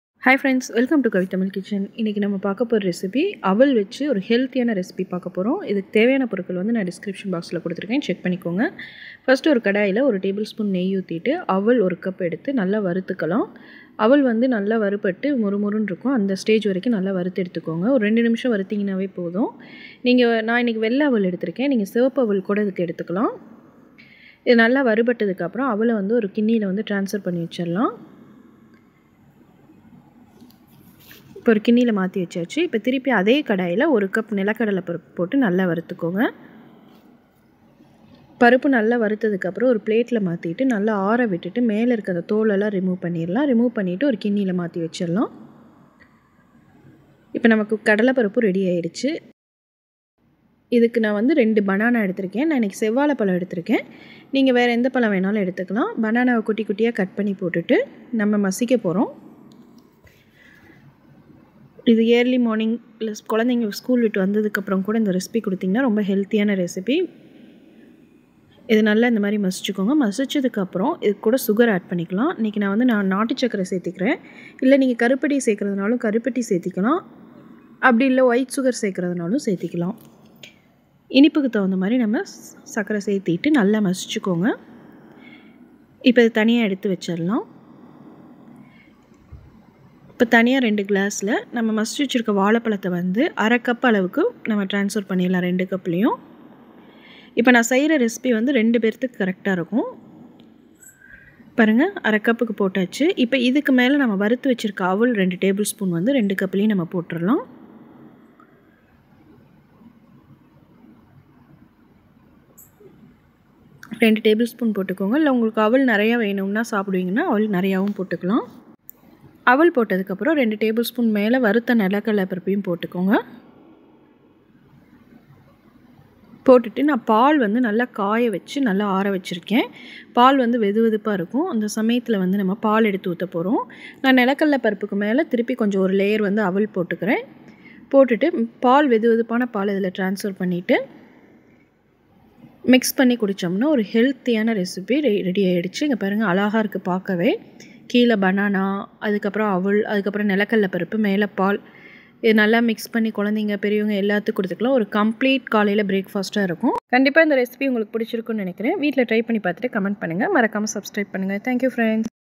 சுலபமாக செய்யலாம் அவல் பால் | Aval Banana milk recipe | Complete breakfast recipe....
Ingredients

Aval 1 cup
Banana 2
Peanut 1 cup
Nattu sakkarai
Milk